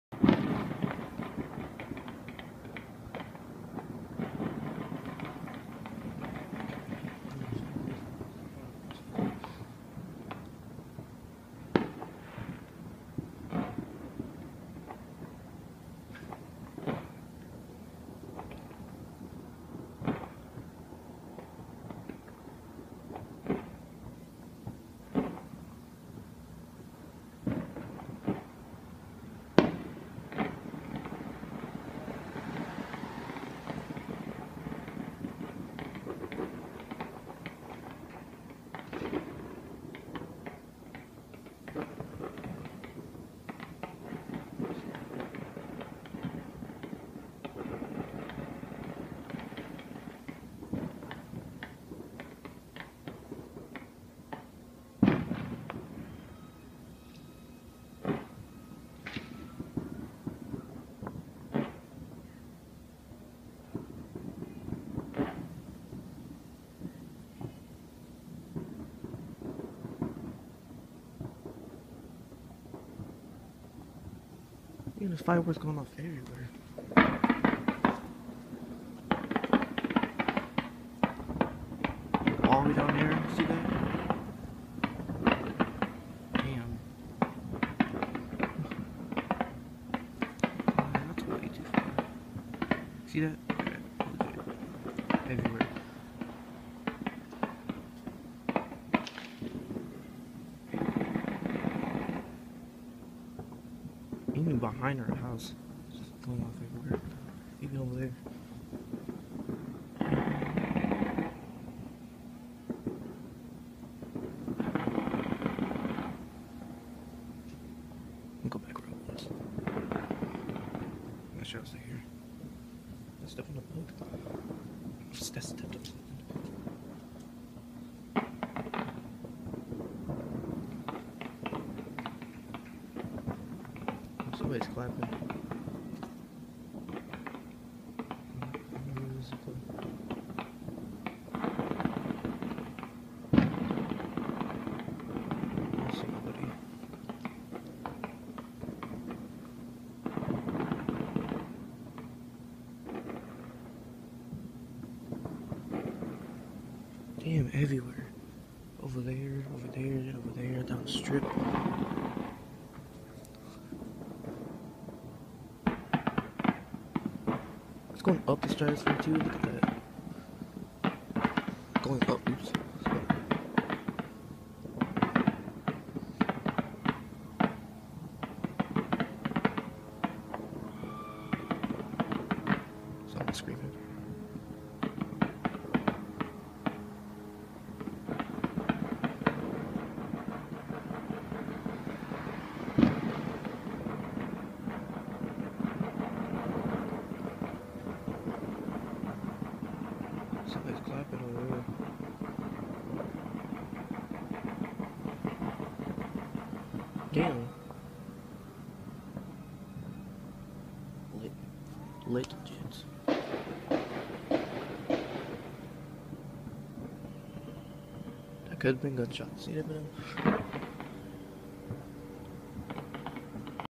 I love Las Vegas on New Years!
This is the city were there is the best fireworks shows in the world (other then New York) lol